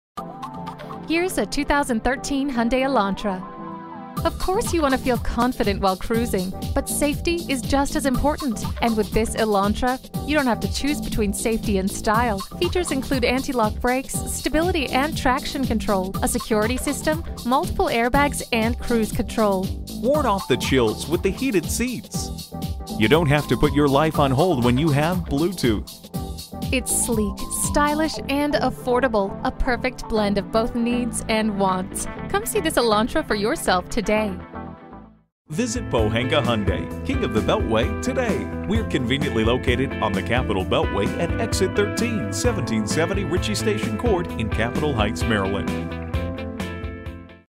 Used 2013 Hyundai Elantra Capitol Heights MD Washington-DC, MD NH3274 - SOLD
Year: 2013
Make: Hyundai
Model: Elantra
Trim: GLS
Engine: 1.8L 4-Cylinder DOHC 16V Dual CVVT
Transmission: 6-Speed
Color: Blue
Interior: Beige
Mileage: 25920
Stock #: NH3274
VIN: KMHDH4AE5DU727107

Address:
1770 Ritchie Station Court